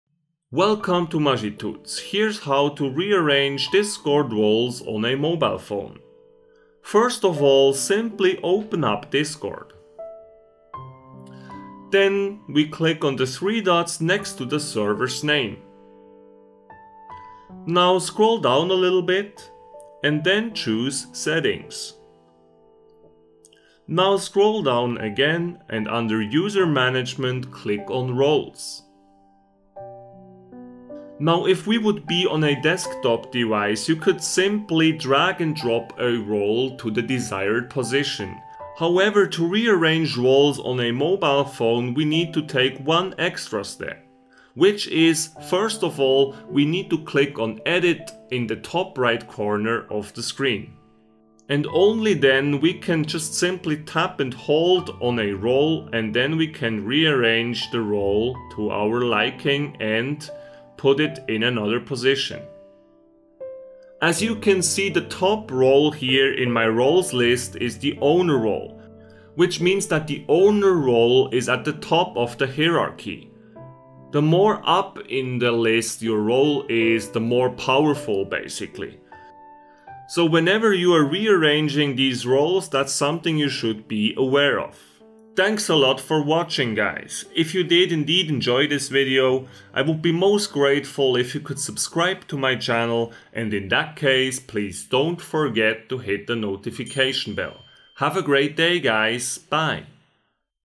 Discord: How To Reorder Roles on Mobile
Here's How to Reorder Roles on Mobile. Enjoy.

0:00 Reorder Roles on Discord Mobile App